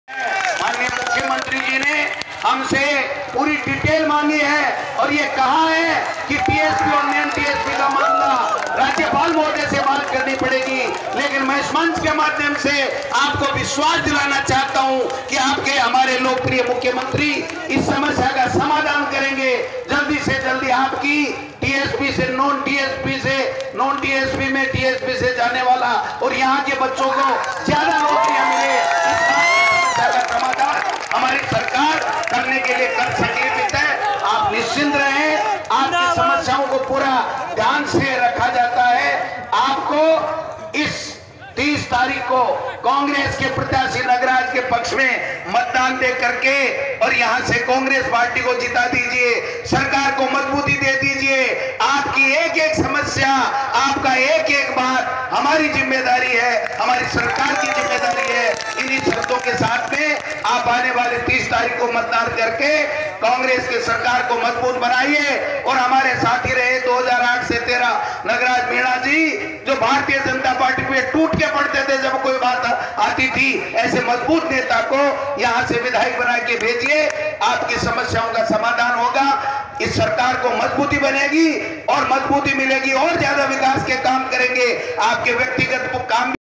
शिक्षा मंत्री गोविंद सिंह डोटासरा //live govind singh dotasara
govind singh dotasara statement on tsp and non tsp teacher transfer